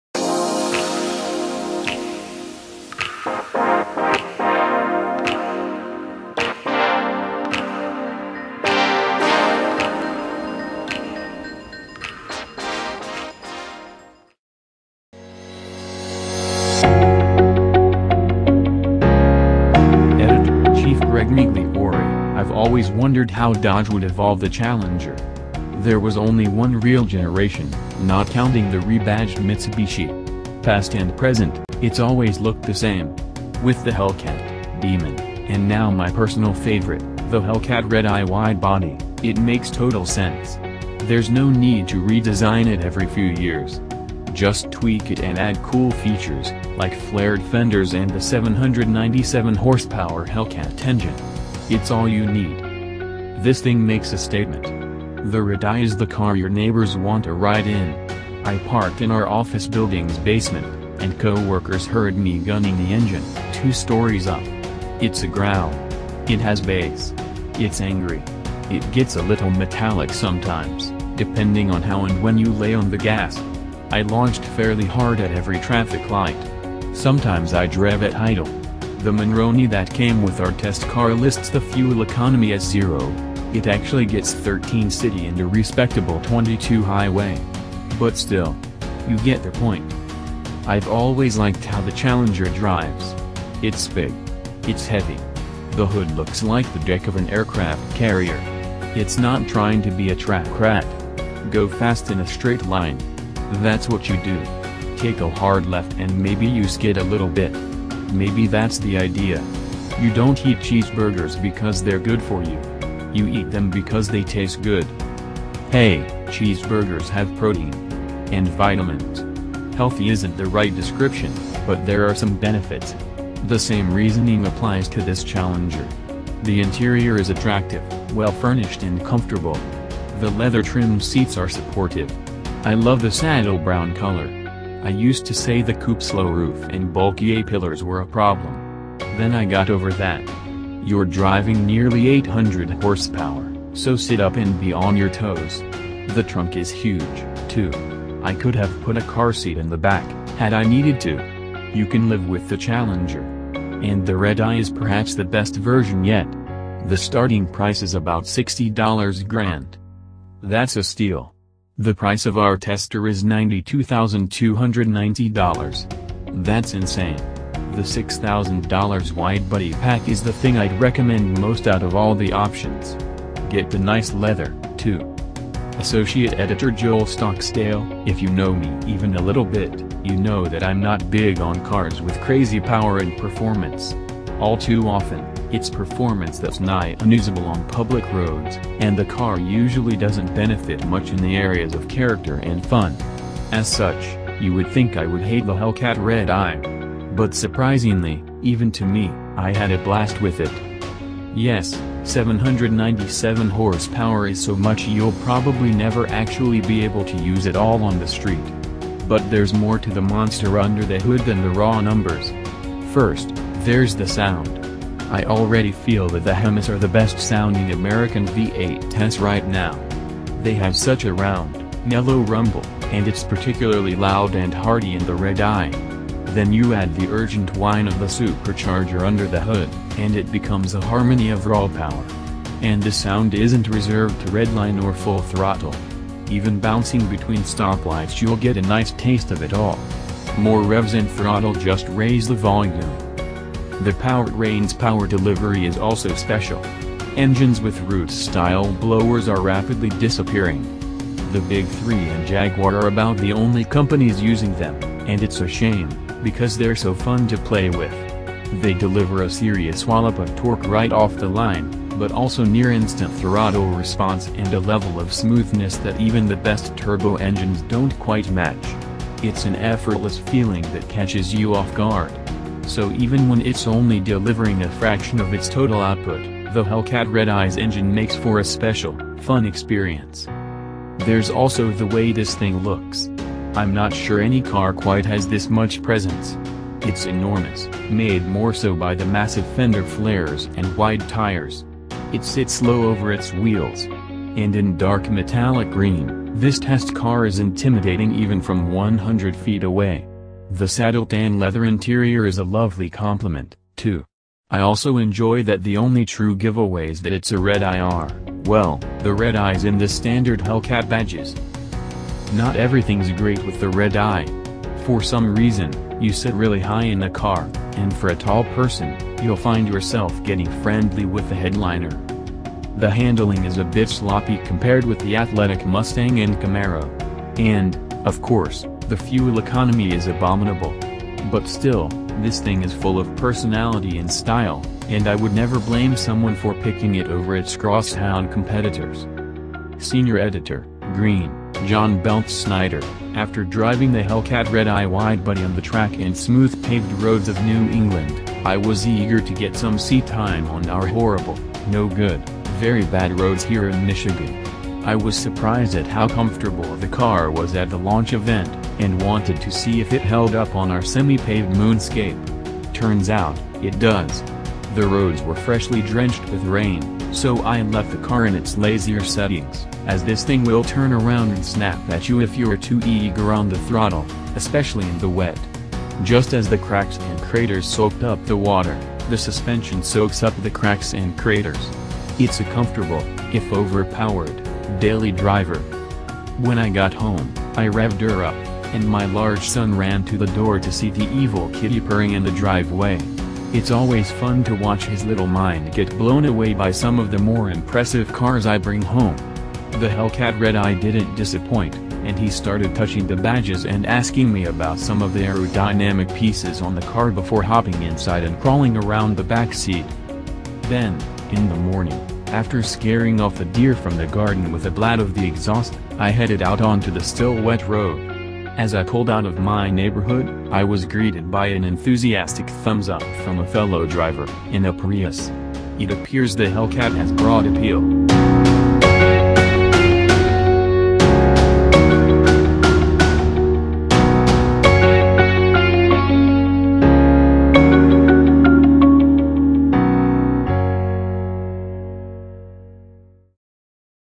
2019 Dodge Challenger SRT Hellcat Redeye Drivers' Notes Wide awake
Editor-in-Chief Greg Migliore — I've always wondered how Dodge would evolve the Challenger. There was only one real generation, not counting the rebadged Mitsubishi. Past and present, it's always looked the same. With the Hellcat, Demon, and now my personal favorite, the Hellcat Redeye Widebody, it makes total sense. There's no need to redesign it every few years. Just tweak it and add cool features — like flared fenders and the 797-hp Hellcat engine. It's all you need.

This thing makes a statement. The Redeye is the car your neighbors want a ride in. I parked in our office building's basement, and co-workers heard me gunning the engine — two stories up. It's a growl. It has bass. It's angry. It gets a little metallic sometimes, depending on how and when you lay on the gas. I launched fairly hard at every traffic light. Sometimes I'd rev at idle. The Monroney that came with our test car lists the fuel economy as zero (it actually gets 13 city and a respectable 22 highway). But still. You get the point.

I've always liked how the Challenger drives. It's big. It's heavy. The hood looks like the deck of an aircraft carrier. It's not trying to be a track rat. Go fast in a straight line. That's what you do. Take a hard left and maybe you skid a little bit. Maybe that's the idea. You don't eat cheeseburgers because they're good for you. You eat them because they taste good.

Hey, cheeseburgers have protein. And vitamins. Healthy isn't the right description, but there are some benefits. The same reasoning applies to this Challenger. The interior is attractive, well-furnished and comfortable. The leather-trimmed seats are supportive. I love the saddle brown color. I used to say the coupe's low roof and bulky A-pillars were a problem. Then I got over that. You're driving nearly 800 hp, so sit up and be on your toes. The trunk is huge, too. I could have put a car seat in the back, had I needed to. You can live with the Challenger. And the Redeye is perhaps the best version yet. The starting price is about $60 grand. That's a steal. The price of our tester is $92,290. That's insane. The $6,000 widebody pack is the thing I'd recommend most out of all the options. Get the nice leather, too.